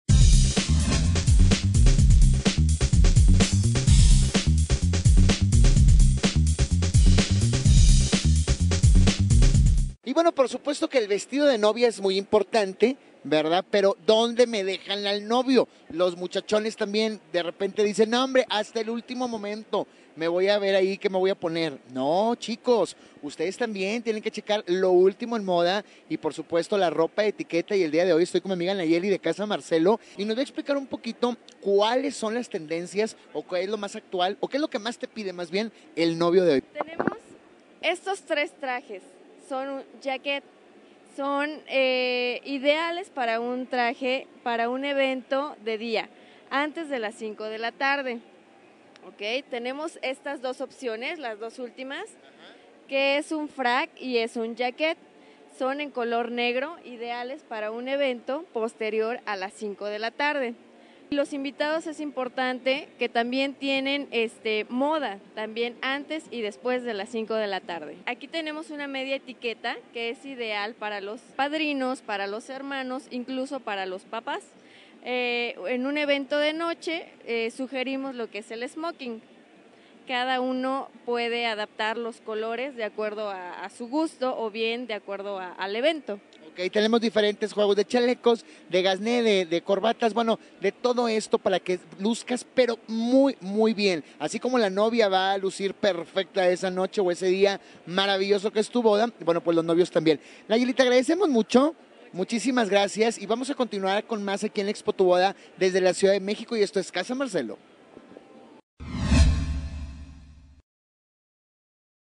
Boda_Novedades en Trajes para el Novio.flv
Conoce las novedades en trajes para el novio el día de su boda.